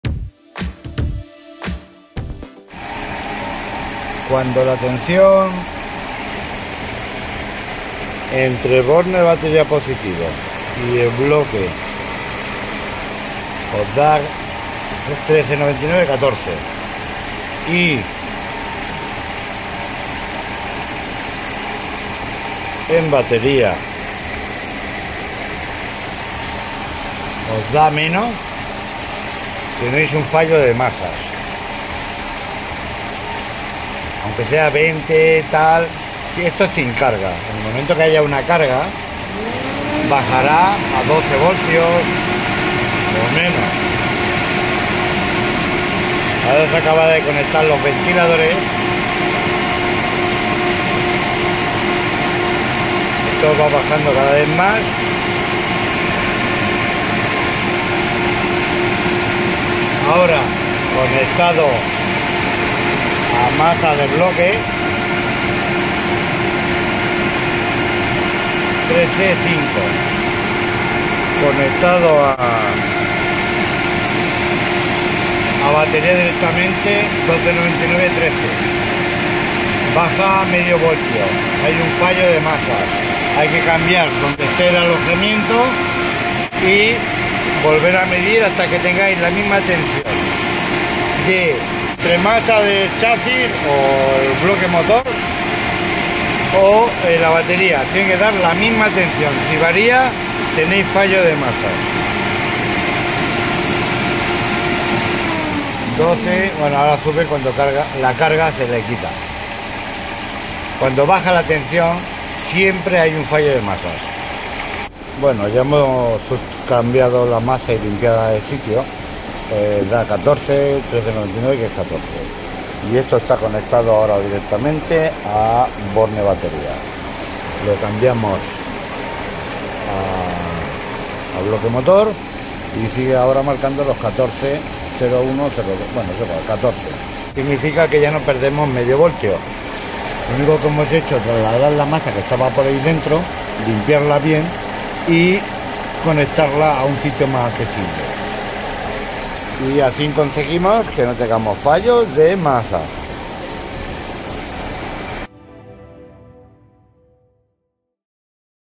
Como saber si tú Lada Niva tiene fallos de masa
Paco, nos explica de forma fácil como conocer si tu Niva tiene fallos de masa. lada ladaniva niva @carlosnivamania